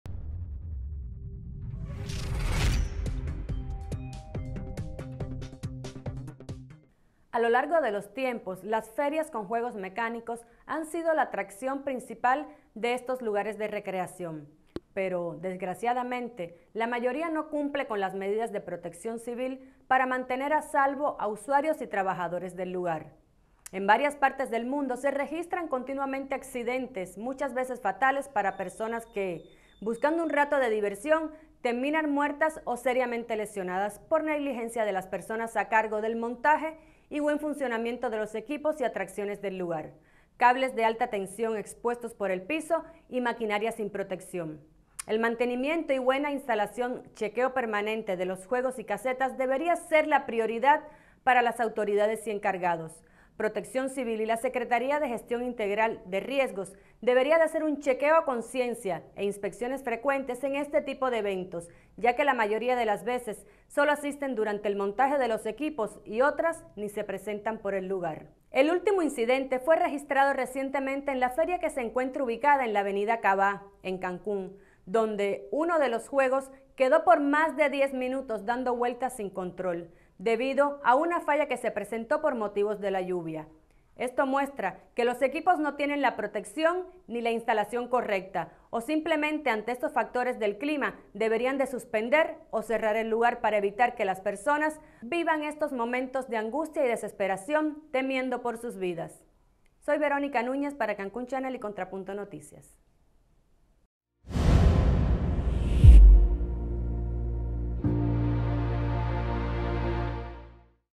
EL TERROR EN LAS FERIAS
No se cumplen las medidas de protección civil en lugares de recreación, causando mortales accidentes